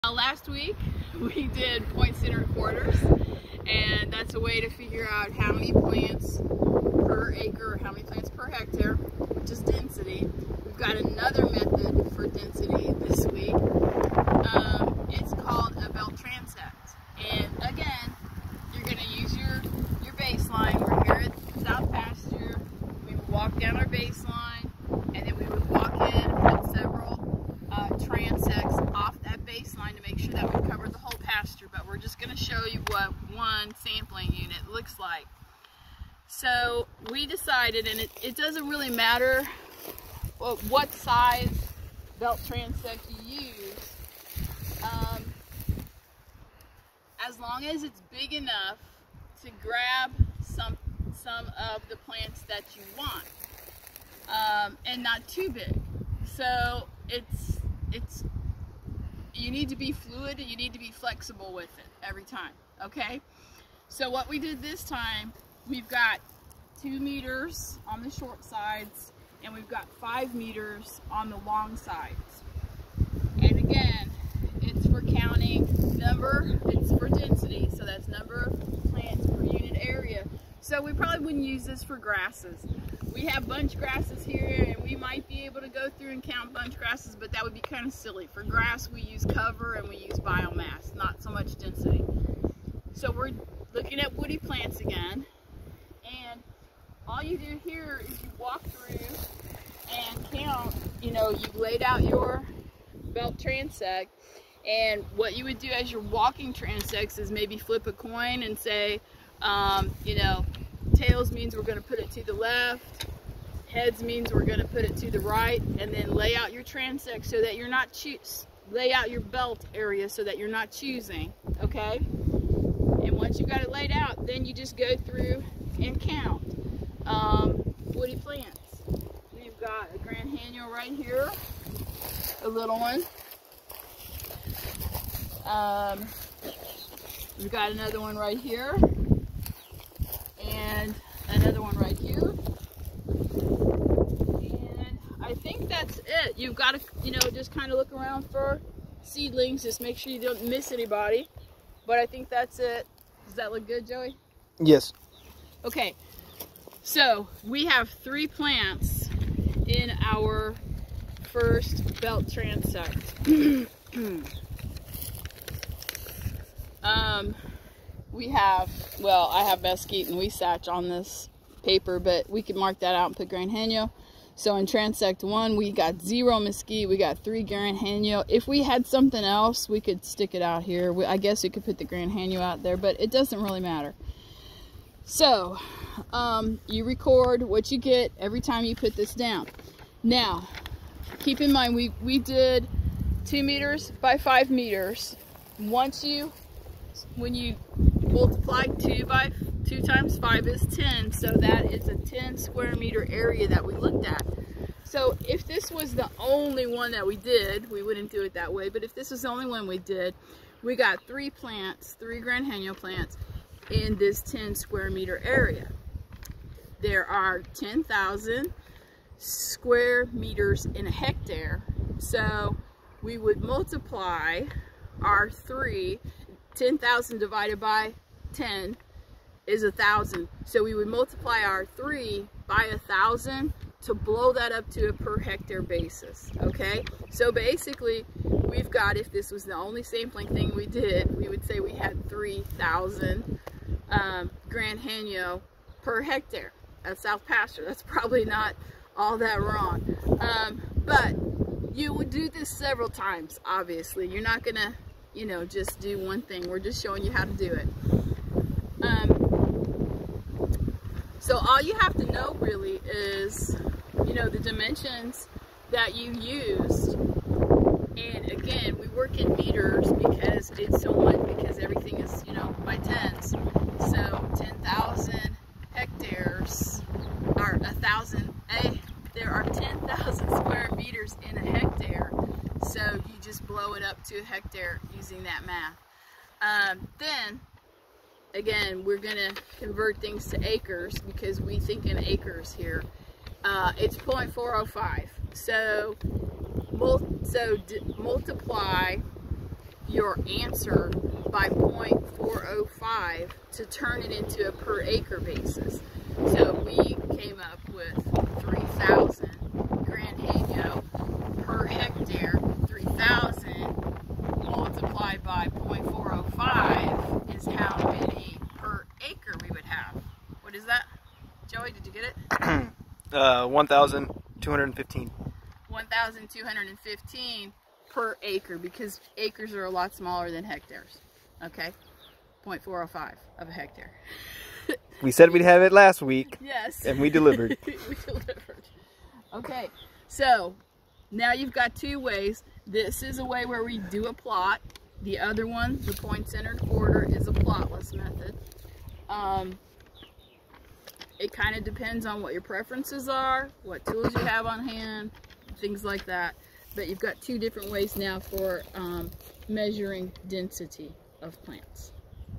Belt Transect sampling technique
An overview of the belt transect field sampling method for plant density